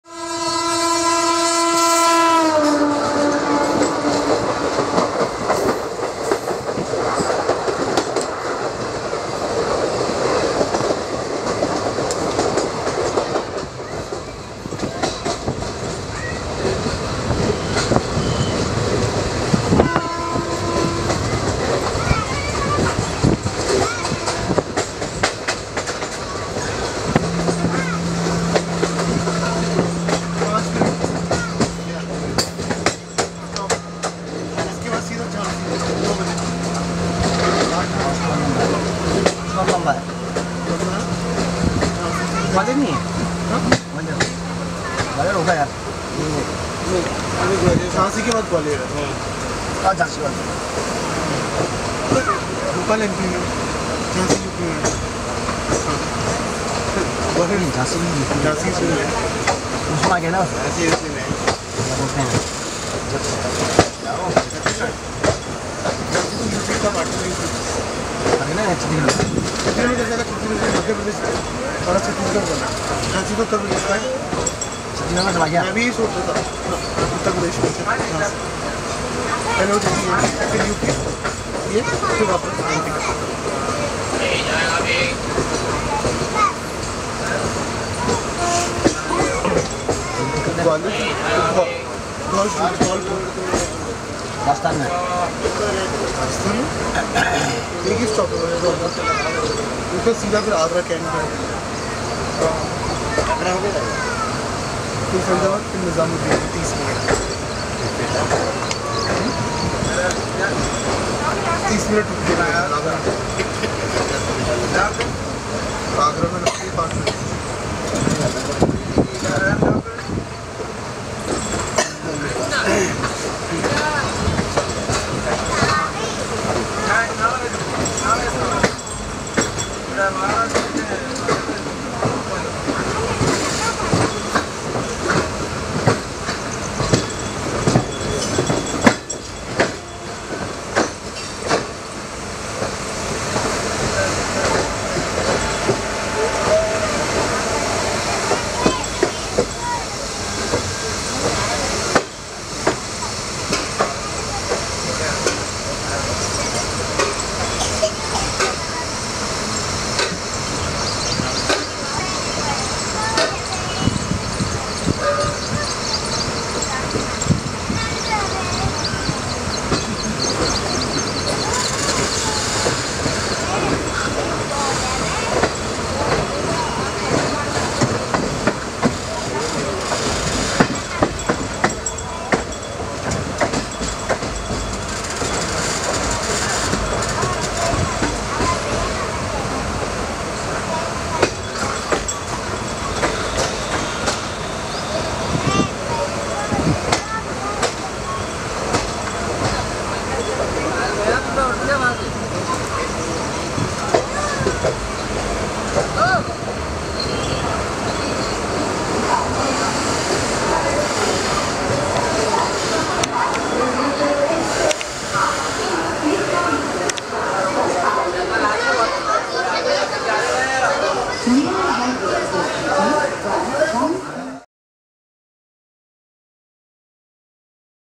Train enters Bhopal Railway Station, India
Bhopal Junction is a major railway junction of India and main railway station of Bhopal, the capital of the central Indian state of Madhya Pradesh. Bhopal junction is the starting point for the fastest train of India,[citation needed] the Bhopal Shatabdi Express, as well as the fastest mail express train which is also India's first ISO certified train, the Shaan-e-Bhopal Express, while the newly announced Bhopal–Indore AC Double Decker Intercity Express will be the first AC Double Decker train[citation needed] and Bhopal–Indore AC Double Decker Non-Stop Express will be the first non-stop point to point train of Central India (Madhya Pradesh and Chhattisgarh). This station also serves as a connecting point for various pilgrims from Nepal, China, Thailand, Malaysia, Japan and Myanmar to visit the Stupa of Sanchi, an important Buddhist stupa, which is about 40 km from Bhopal Junction railway station.

Source: Wikipedia